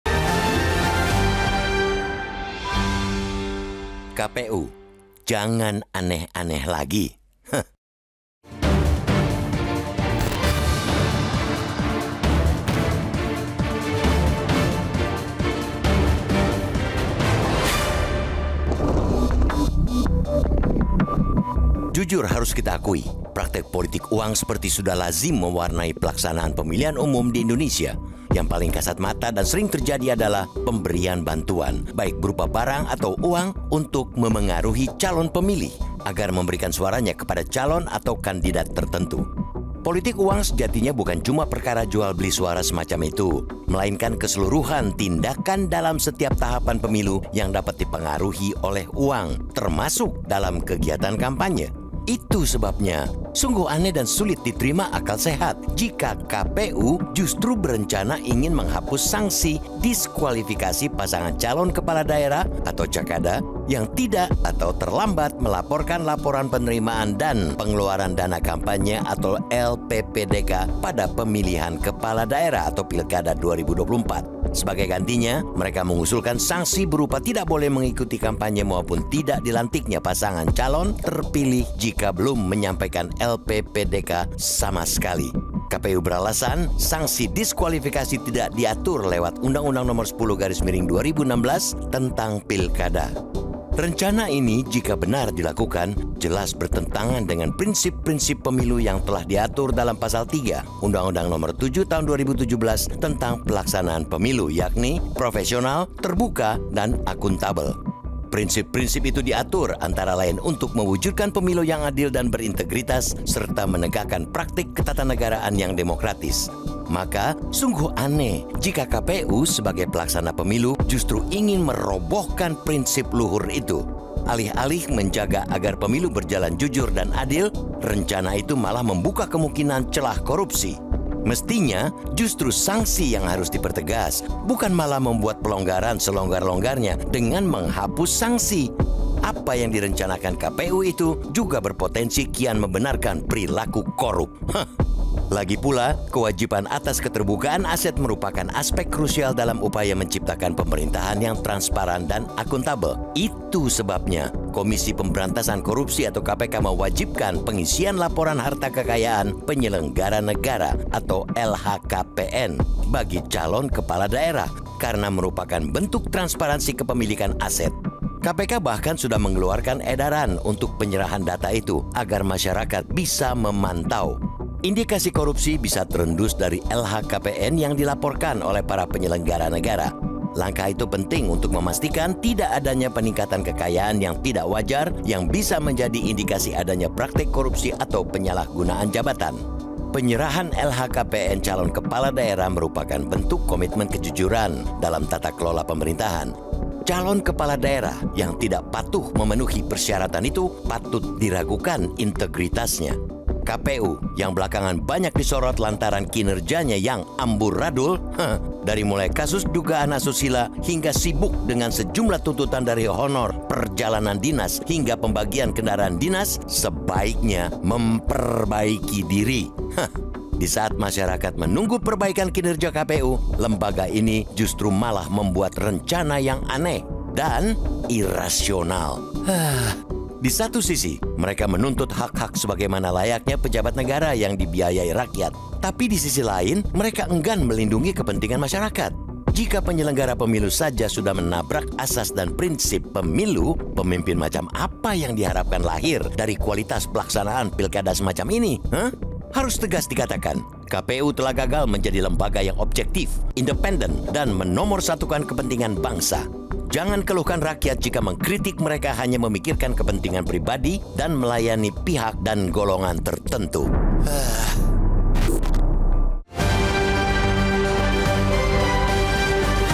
KPU Jangan Aneh-aneh Lagi
MetroTV, JUJUR harus kita akui, praktik politik uang seperti sudah lazim mewarnai pelaksanaan pemilihan umum di Indonesia. Yang paling kasat mata dan sering terjadi adalah pemberian bantuan, baik berupa barang atau uang untuk memengaruhi calon pemilih agar memberikan suaranya kepada calon atau kandidat tertentu.

Politik uang sejatinya bukan cuma perkara jual beli suara semacam itu, melainkan keseluruhan tindakan dalam setiap tahapan pemilu yang dapat dipengaruhi oleh uang, termasuk dalam kegiatan kampanye.

Itu sebabnya, sungguh aneh dan sulit diterima akal sehat jika Komisi Pemilihan Umum (KPU) justru berencana ingin menghapus sanksi diskualifikasi pasangan calon kepala daerah (cakada), yang tidak atau terlambat melaporkan Laporan Penerimaan dan Pengeluaran Dana Kampanye (LPPDK) pada Pemilihan Kepala Daerah (Pilkada) 2024.

Sebagai gantinya, mereka mengusulkan sanksi berupa tidak boleh mengikuti kampanye maupun tidak dilantiknya pasangan calon terpilih jika belum menyampaikan LPPDK sama sekali. KPU beralasan, sanksi diskualifikasi tidak diatur lewat Undang-Undang Nomor 10/2016 tentang Pilkada.

Rencana ini, jika benar dilakukan, jelas bertentangan dengan prinsip-prinsip pemilu yang telah diatur dalam pasal 3 UU Nomor 7 Tahun 2017 tentang Pelaksanaan Pemilu, yakni profesional, terbuka, dan akuntabel. Prinsip-prinsip itu diatur antara lain untuk mewujudkan pemilu yang adil dan berintegritas serta menegakkan praktik ketatanegaraan yang demokratis.

Maka, sungguh aneh jika KPU, sebagai pelaksana pemilu, justru ingin merobohkan prinsip luhur itu. Alih-alih menjaga agar pemilu berjalan jujur dan adil, rencana itu malah membuka kemungkinan celah korupsi. Mestinya, justru sanksi yang harus dipertegas, bukan malah membuat pelonggaran selonggar-longgarnya dengan menghapus sanksi. Apa yang direncanakan KPU itu juga berpotensi kian membenarkan perilaku korup.

Lagi pula, kewajiban atas keterbukaan aset merupakan aspek krusial dalam upaya menciptakan pemerintahan yang transparan dan akuntabel. Itu sebabnya, Komisi Pemberantasan Korupsi (KPK) mewajibkan pengisian laporan harta kekayaan penyelenggara negara (LHKPN) bagi calon kepala daerah, karena merupakan bentuk transparansi kepemilikan aset. KPK bahkan sudah mengeluarkan edaran untuk penyerahan data itu, agar masyarakat bisa memantau.

Indikasi korupsi bisa terendus dari LHKPN yang dilaporkan oleh para penyelenggara negara. Langkah itu penting untuk memastikan tidak adanya peningkatan kekayaan yang tidak wajar, yang bisa menjadi indikasi adanya praktik korupsi atau penyalahgunaan jabatan.

Penyerahan LHKPN calon kepala daerah merupakan bentuk komitmen kejujuran dalam tata kelola pemerintahan. Calon kepala daerah yang tidak patuh memenuhi persyaratan ini, patut diragukan integritasnya.

KPU, yang belakangan banyak disorot lantaran kinerjanya yang amburadul, dari mulai kasus dugaan asusila, hingga sibuk dengan sejumlah tuntutan dari honor perjalanan dinas hingga pembagian kendaraan dinas, sebaiknya memperbaiki diri. Di saat masyarakat menunggu perbaikan kinerja KPU, lembaga ini justru malah membuat rencana yang aneh dan irasional.

Di satu sisi, mereka menuntut hak-hak sebagaimana layaknya pejabat negara yang dibiayai rakyat, tapi di sisi lain mereka enggan melindungi kepentingan masyarakat. Jika penyelenggara pemilu saja sudah menabrak asas dan prinsip pemilu, pemimpin macam apa yang diharapkan lahir dari kualitas pelaksanaan pilkada semacam ini?

Harus tegas dikatakan, KPU telah gagal menjadi lembaga yang objektif, independen, dan menomorsatukan kepentingan bangsa. Jangan keluhkan rakyat jika mengkritik mereka hanya memikirkan kepentingan pribadi dan melayani pihak dan golongan tertentu.